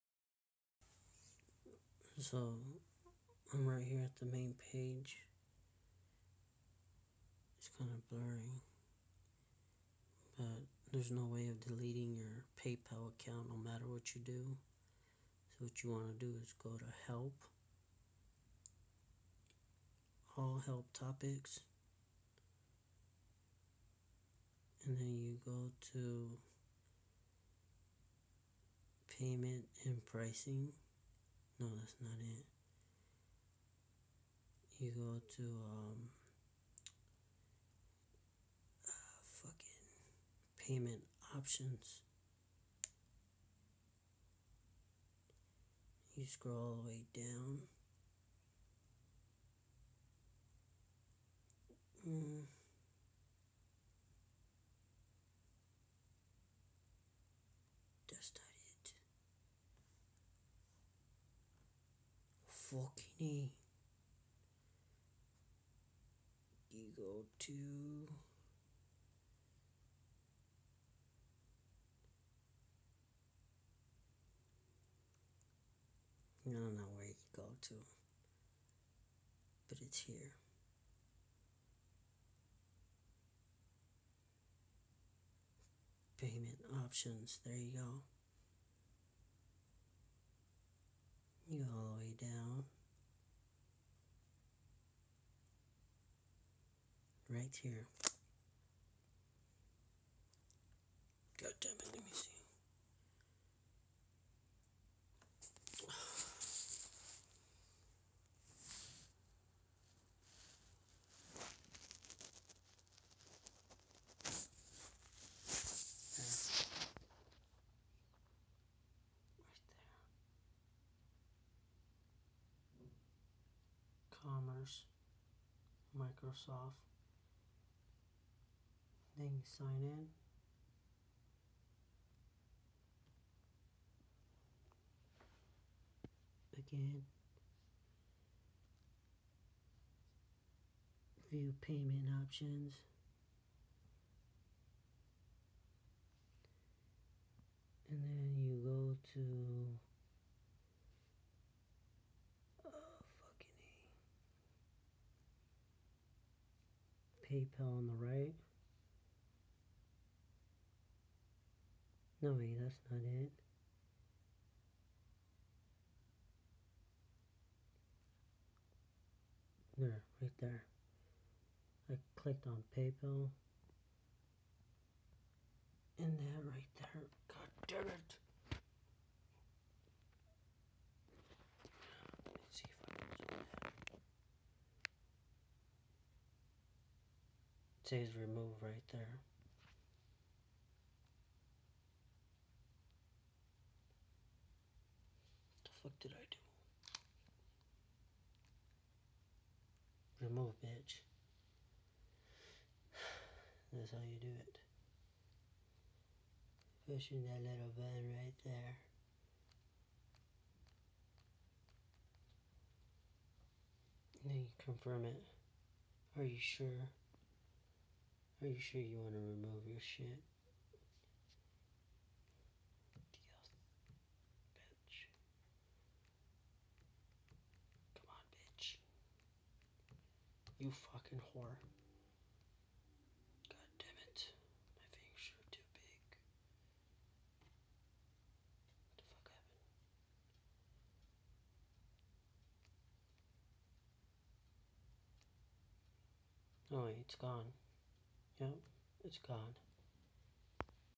Remove PayPal from microsoft
Sorry it's a bad video but basicly go to help then more help topics